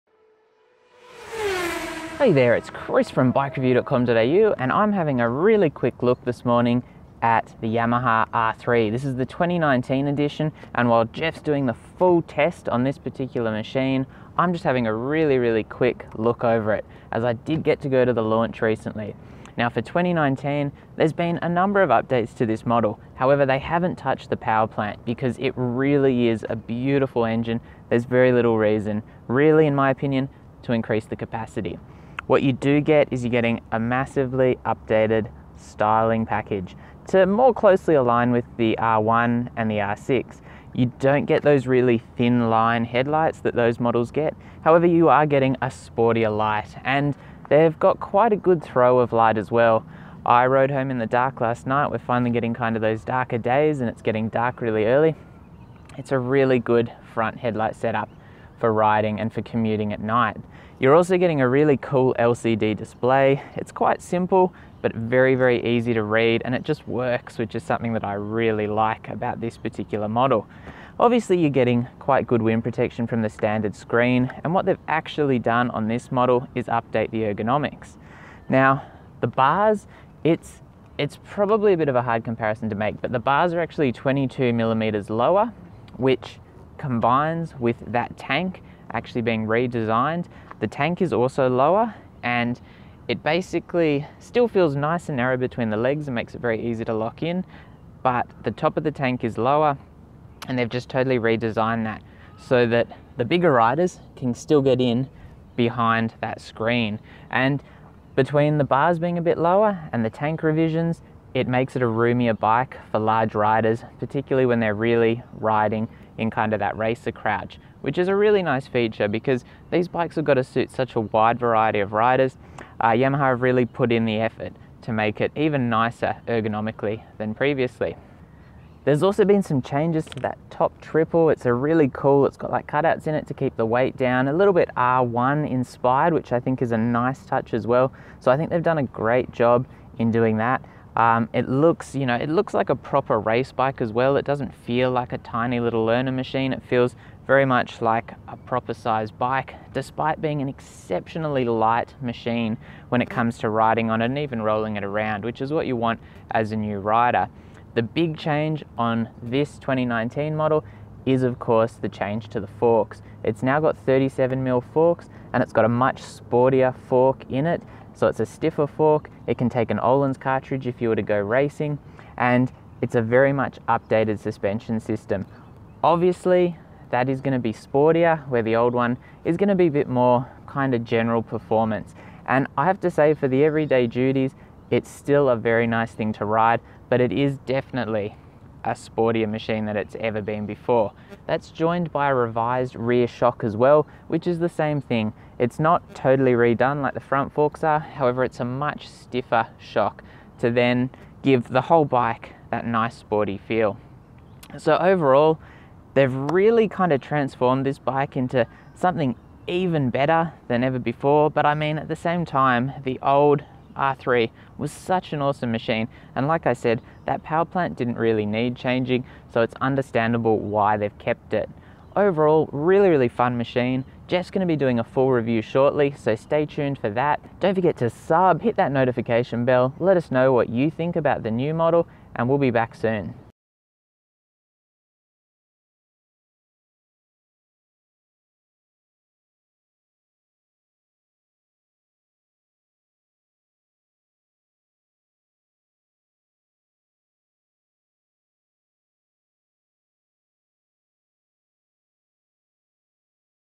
2019 Yamaha YZF-R3 - First Look & Updates
Yamaha have updated their flagship learner sportsbike model for 2019, and while there's been some significant updates, they've retained the 321cc parallel twin cylinder powerplant. Here's a full look at what's new, updated and improved.